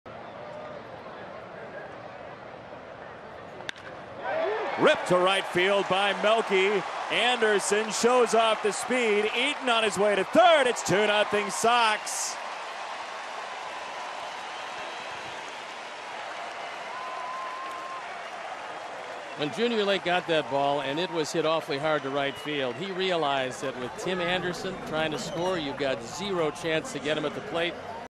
TOR@CWS: Cabrera brings home Anderson on a liner
Melky Cabrera knocks a line-drive single into right field, allowing Tim Anderson to score from second to give the White Sox a 2-0 lead